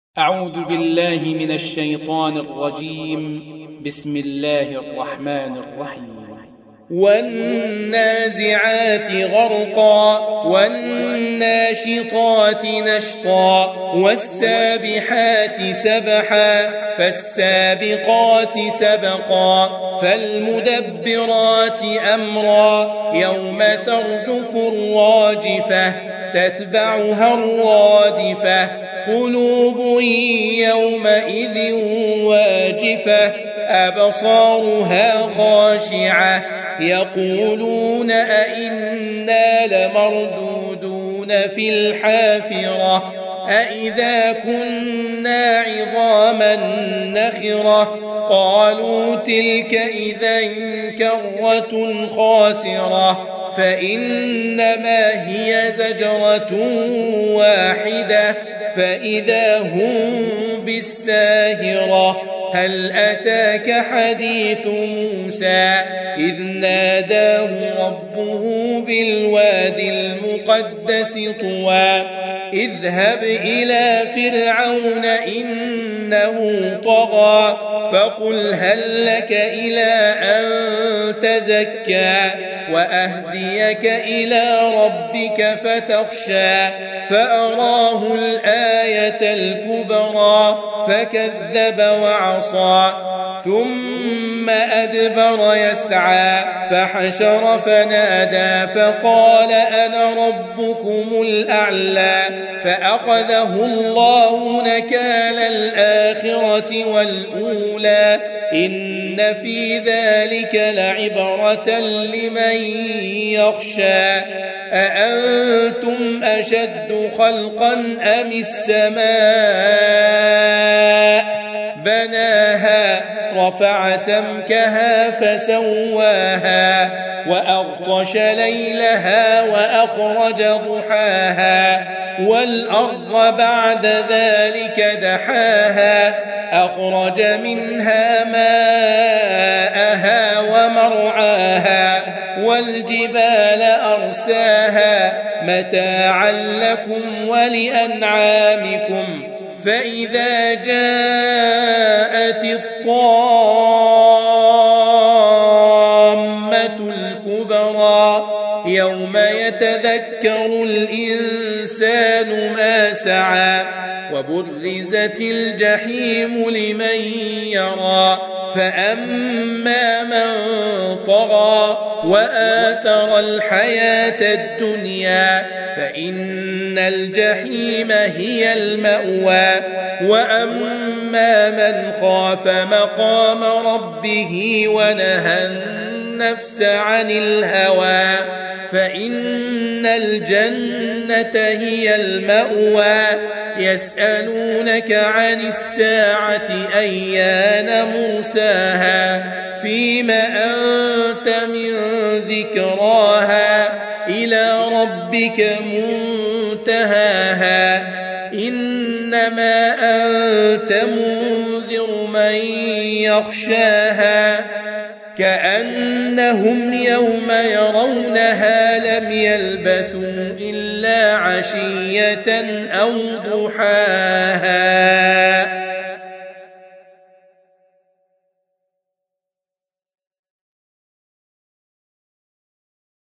Surah An-Nazi'at (Those Who Drag Forth) Syed Ramadan Quran Recitation
Beautiful Quran Recitation of Surah An-Nazi'at (Those Who Drag Forth) by Syed Ramadan